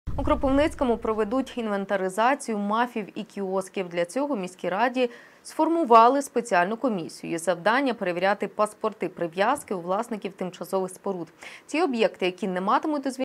У Кропивницькому демонтують МАФи, які встановлені без дозволів
У Кропивницькому проведуть інвентаризацію МАФів та кіосків. Для цього в міській раді сформували спеціальну комісію. Її завдання – перевіряти паспорти прив’язки у власників тимчасових споруд. Ті об’єкти, які не матимуть дозвільних документів або їхній термін сплив, демонтують. Продовжити строк дії паспортів прив’язки власники МАФів та інших тимчасових споруд можуть у Центрі надання адміністративних послуг.